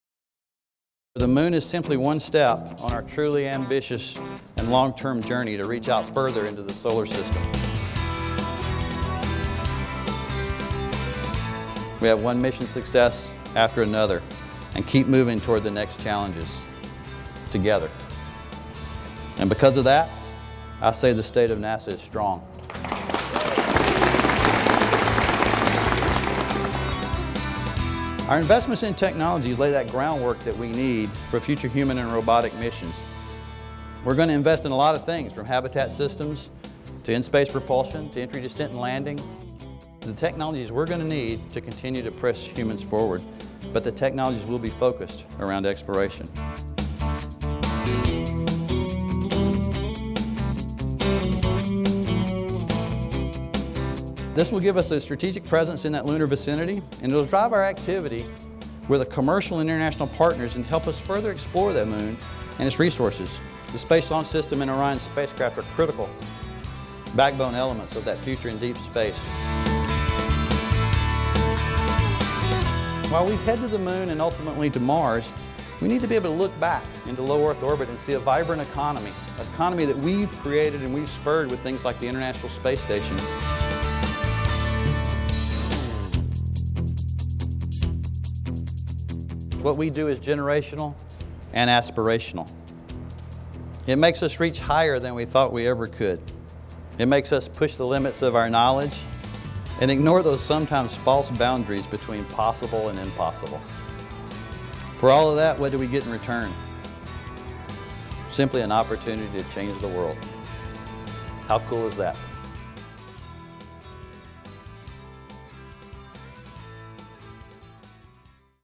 State of NASA: How Cool Is That?
On Feb. 12, NASA centers across the country hosted “State of NASA” events, following President Trump’s Fiscal Year 2019 budget proposal delivery to the U.S. Congress. The events included an address, by acting NASA Administrator Robert Lightfoot, to the agency’s workforce, from NASA’s Marshall Space Flight Center, in Huntsville, Alabama. During his speech, Lightfoot highlighted how the budget would help the agency achieve its goals for space exploration.